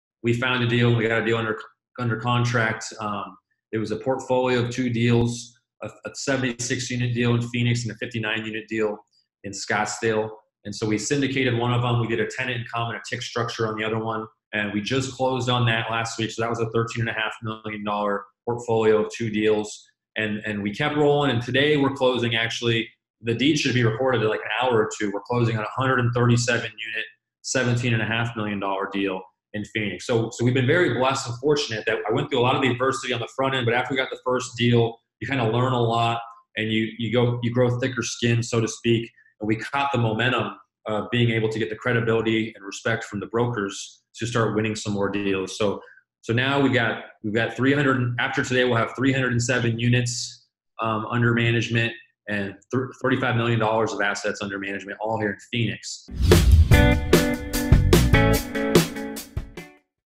Zach Haptonstall ZH Multifamily preview
Zach briefly discusses how he built a portfolio of $35M in multifamily assets at the age of 27 years old.